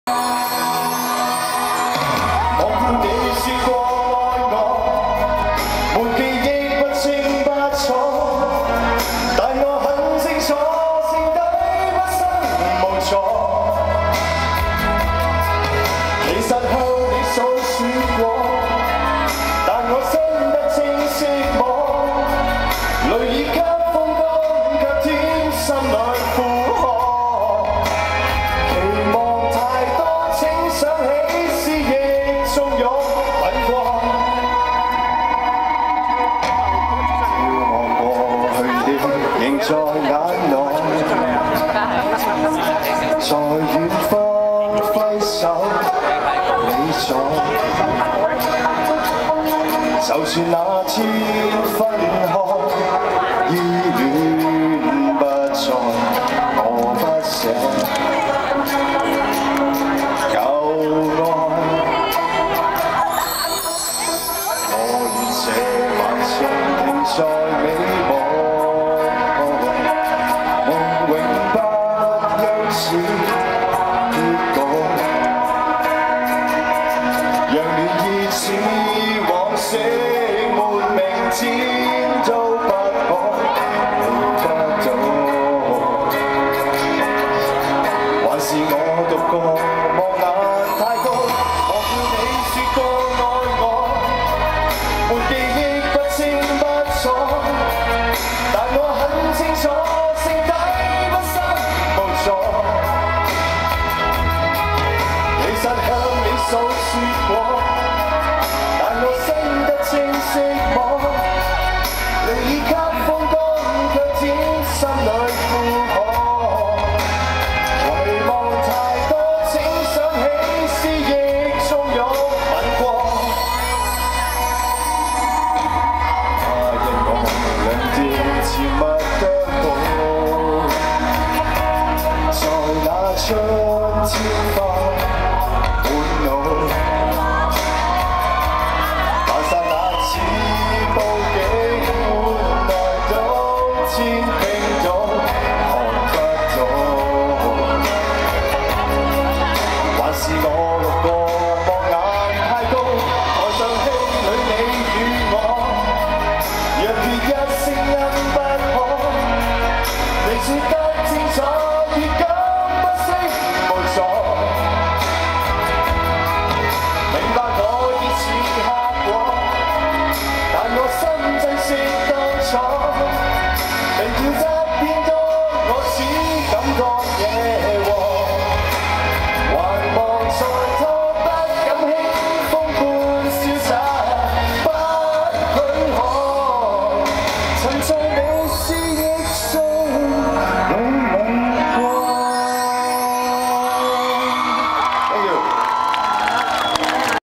Ron Ng @ Chin Picnic
Second song that he sang @ Chin Picnic 2008 anyone know wat it's called?